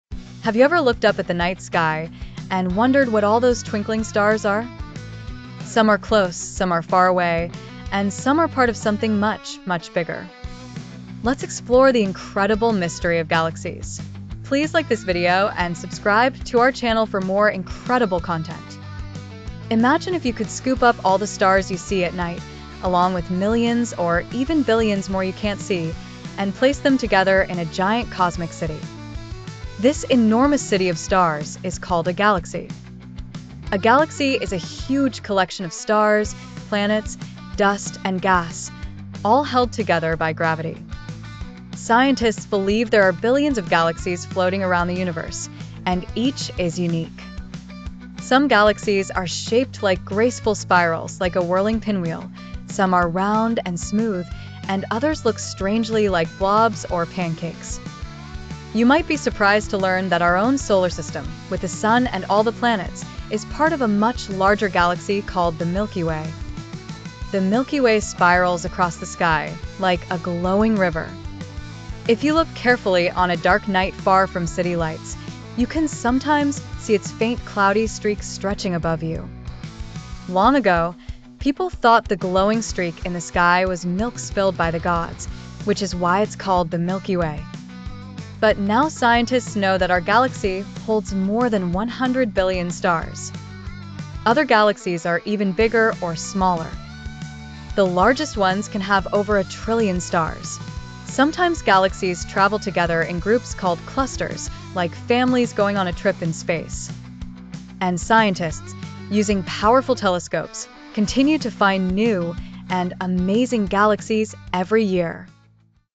Exploring the Wonders of Galaxies
This narrative invites readers to explore the fascinating world of galaxies, vast collections of stars, planets, dust, and gas held together by gravity. It highlights the uniqueness of different galaxies, including our own Milky Way, and explains how they come in various shapes and sizes. The text encourages curiosity about the universe, emphasizing that each star we see is part of a larger cosmic neighborhood filled with mysteries yet to be uncovered.